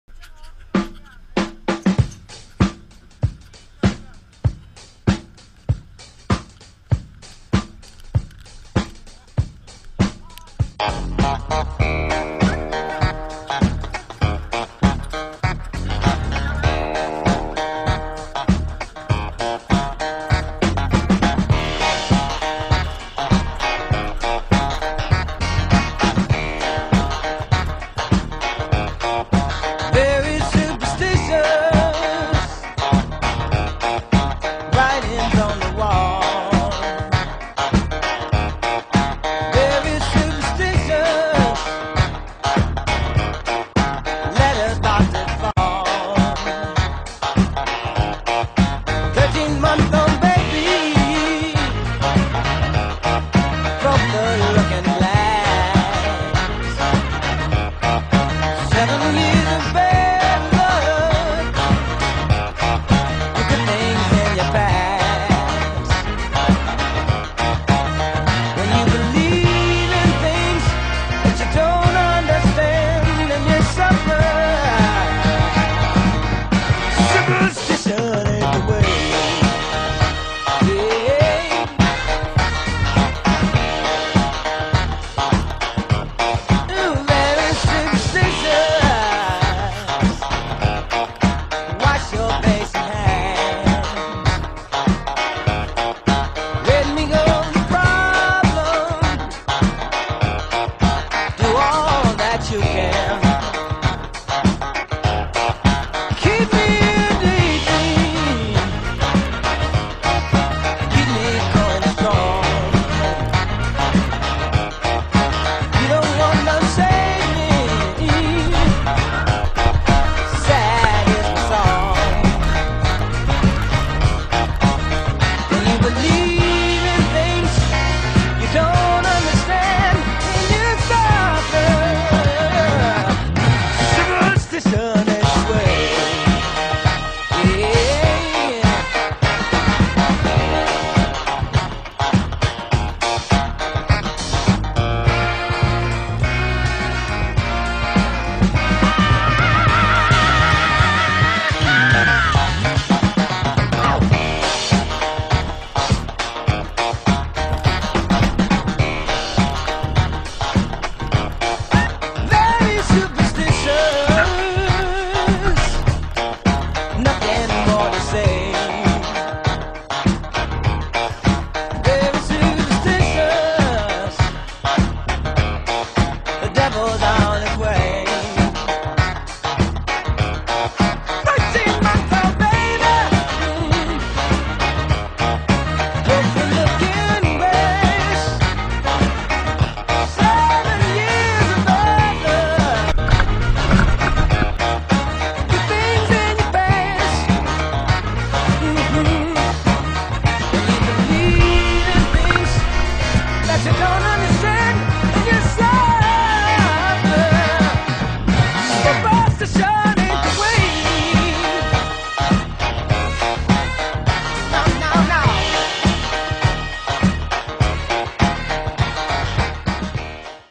Ski Ward Minipark Sesh
With all the snow, the regular terrain park was outrageously slow.  But the show must go on, the team hiked over to the other side of the hill to hit up a couple little features.

Skiers:
Parker Bull
Jared Bull
Jamie Bechta
Storm McStormy
Josh
Lily (snowboarders)